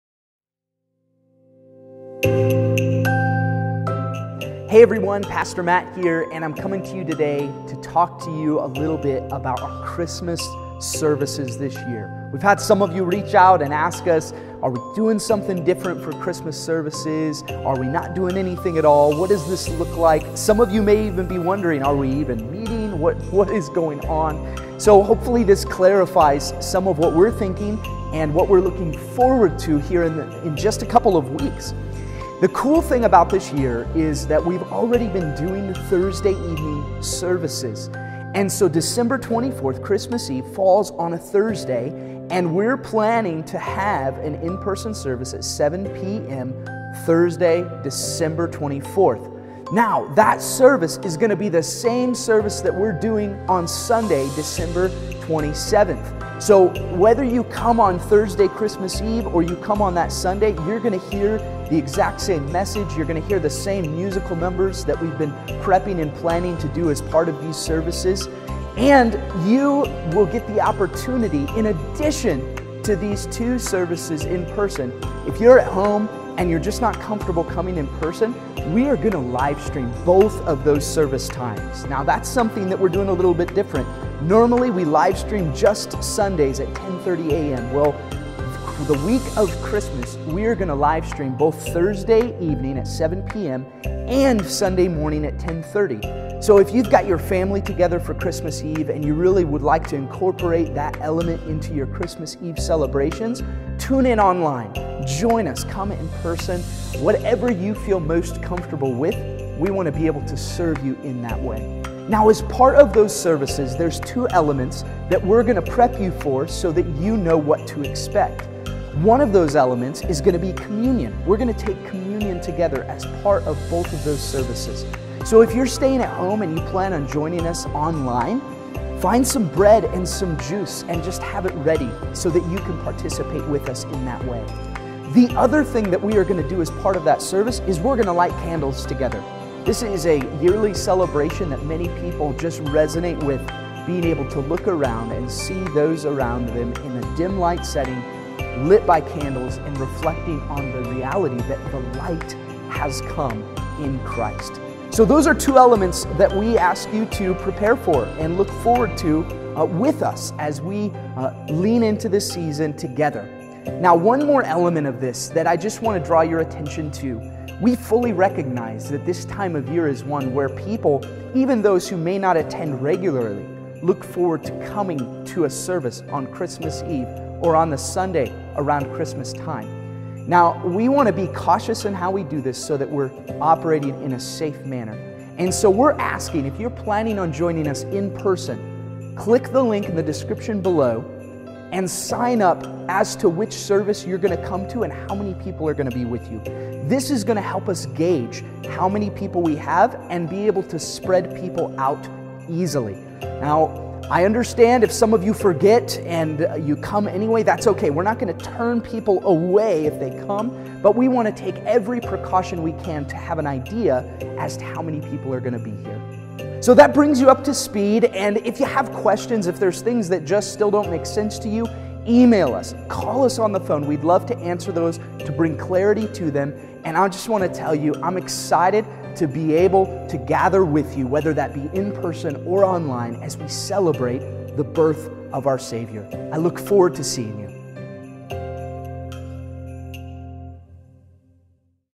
Christmas Services Video
Pastor Matt with some info and clarification regarding our Christmas week services. Pass it along!